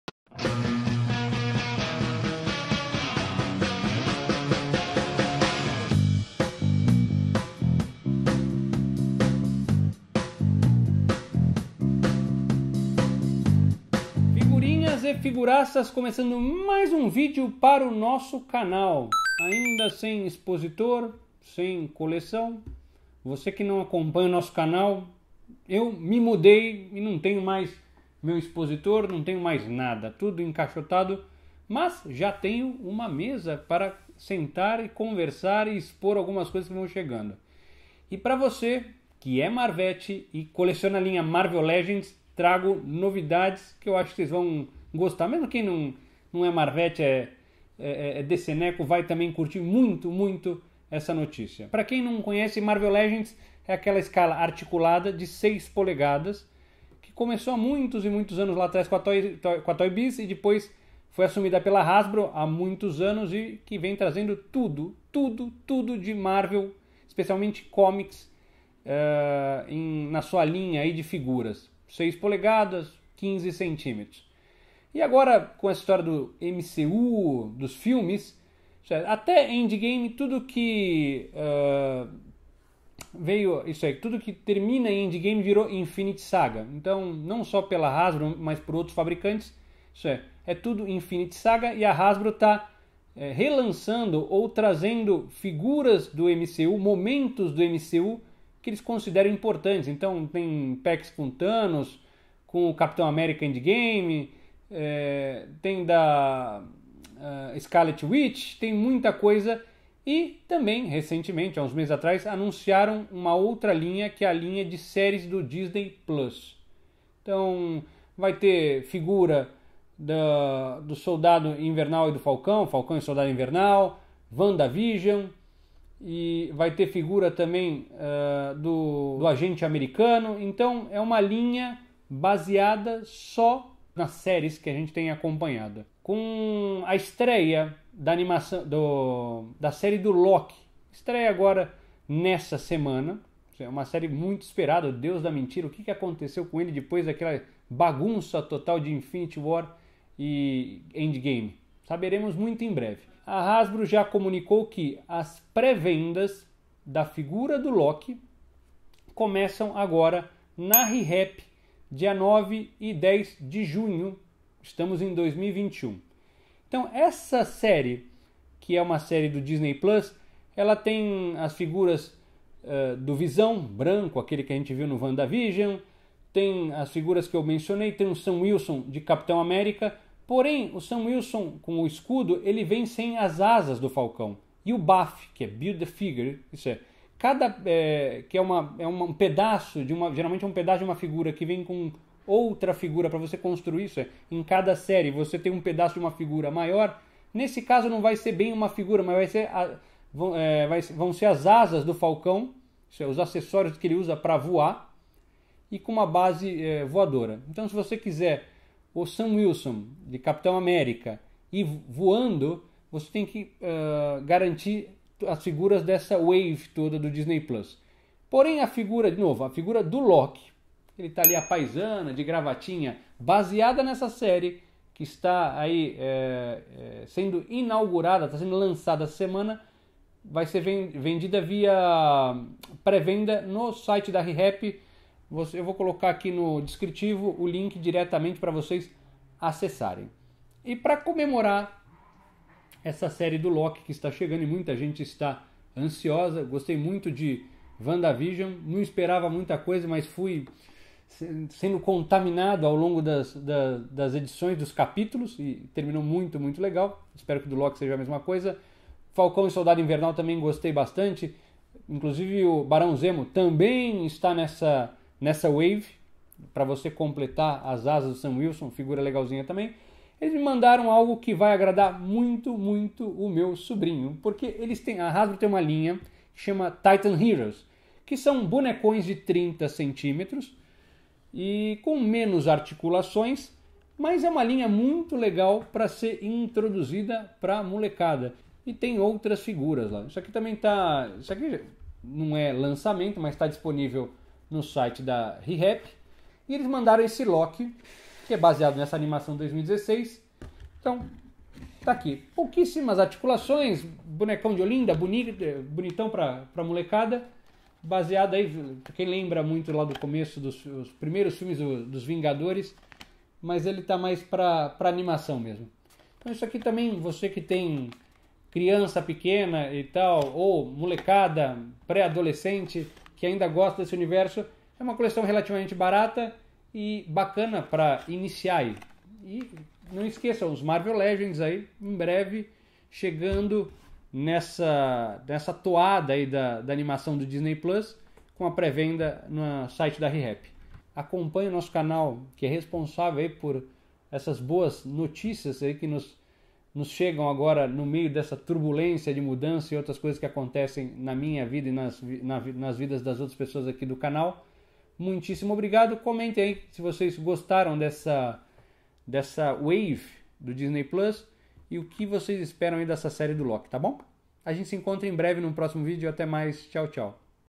FIGURAÇAS #416: MARVEL LEGENDS (Disney Plus), E PRÉVENDA LOKI.
Com a chegada da nova série do Disney Plus, relacionada ao Universo Marvel nos cinemas, e dessa vez abordando o deus da mentira, Loki, a Hasbro inicia oficialmente as vendas da wave relacionada, junto à coleção Marvel Legends, no modelo de pré-venda pela Ri Happy.

Participações em nosso espaço, para comentários e memes, sempre são bem-vindas! Não deixe de expressar sua opinião ou "causo" sobre o tema.